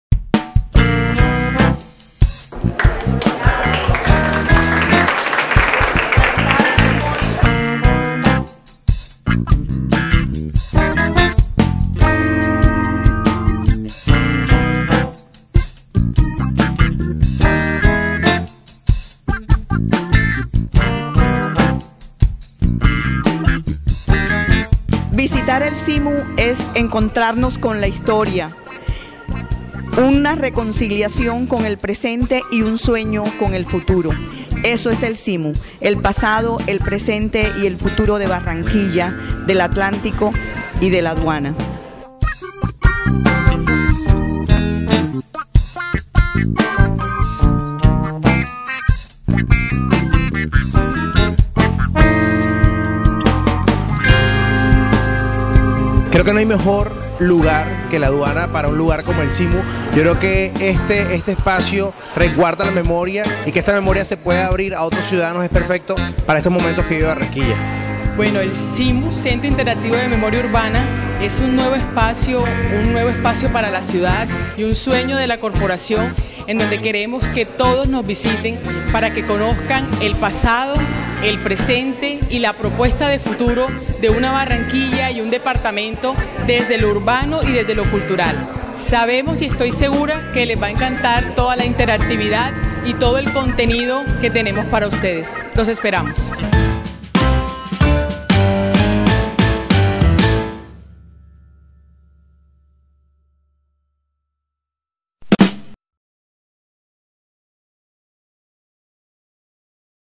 Inauguración del Centro Interactivo de Memoria Urbana- CIMU en Barranquilla.
El CIMU es un proyecto que se redefine hacia el futuro como espacio patrimonial, como centro cultural y como lugar de interés turístico.
Conoce más de el en cimu.com.co